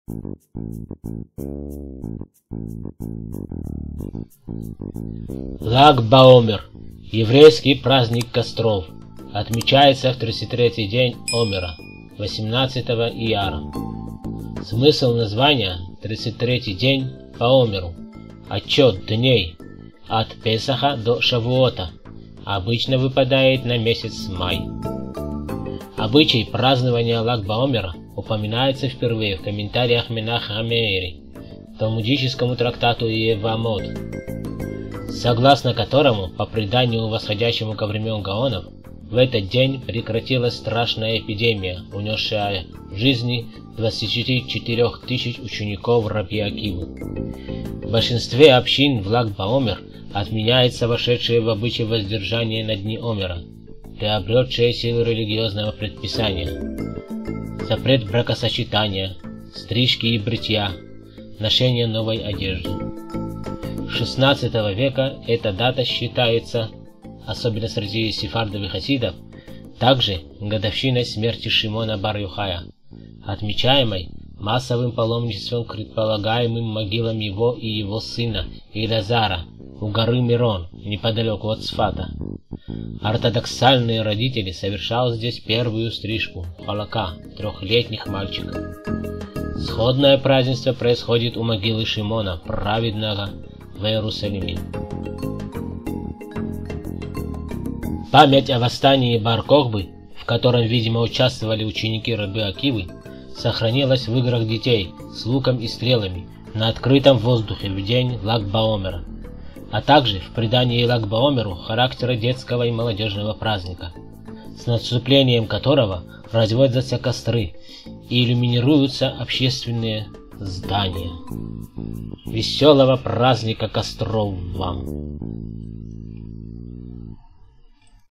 Лаг ба Омер Еврейский праздник костров Израиль
Лаг ба-Омер   (ивр. לַ״ג בָּעוֹמֶר) — еврейский праздник костров. Отмечается в 33-й день Омера (18 ияра). Смысл названия: 33-й день по Омеру, счёту дней от Песаха до Шавуота. обычно выпадает на месяц Май
История праздника
Обычай празднования Лаг ба-‘омера упоминается впервые в комментариях Менахема Меири к талмудическому трактату Иевамот, согласно которому (по преданию, восходящему ко времени гаонов) в этот день прекратилась страшная эпидемия, унесшая многих из 24 тыс. учеников рабби Акивы

В большинстве общин в Лаг ба-‘омер отменяется вошедшее в обычай воздержание на дни омера, приобретшее силу религиозного предписания (запрет бракосочетания , стрижки и бритья, ношения новой одежды; ). С 16 в. эта дата считается (особенно среди сефардов и хасидов ) также годовщиной смерти Шим‘она бар Иохая, отмечаемой массовым паломничеством к предполагаемым могилам его и его сына Эл‘азара у горы Мерон неподалеку от Цфата. Ортодоксальные родители совершают здесь первую стрижку (халака) трехлетних мальчиков. Сходное празднество происходит у могилы Шим‘она Праведного в Иерусалиме.

Память о восстании Бар-Кохбы, в котором, видимо, участвовали ученики рабби Акивы, сохранилась в играх детей с луком и стрелами на открытом воздухе в день Лаг ба-‘омер, а также в придании Лаг ба-‘омеру характера детского и молодежного праздника , с наступлением которого разводятся костры и иллюминируются общественные здания.

Веселого праздника костров вам